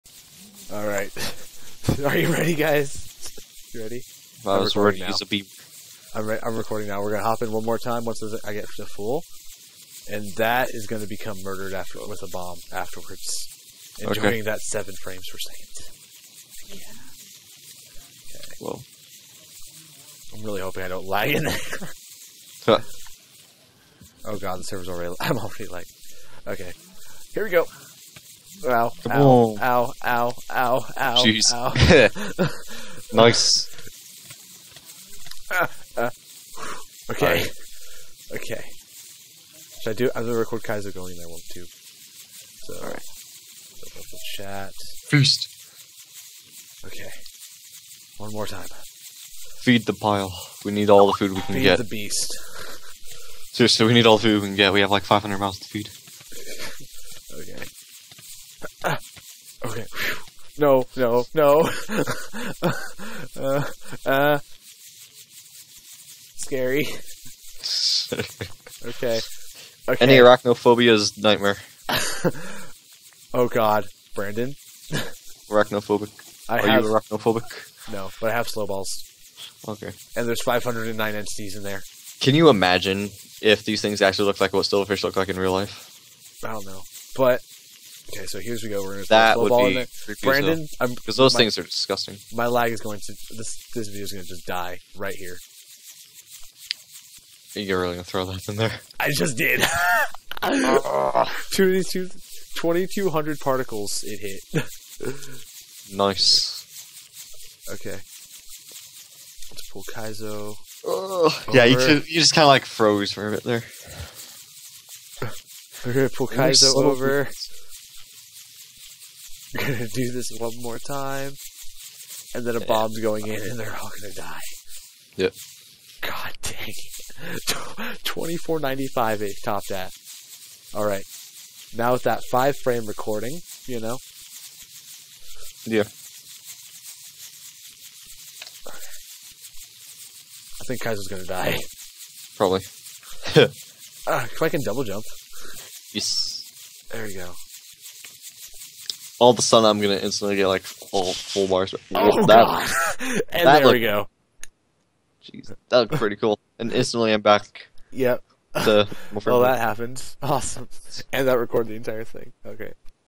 Screwing Around with Silverfish
TheLetsPlayGhost and I decided to screw around with Silverfish, Polerex11 helped some then he lagged out.
This was the end result, ENJOY!!!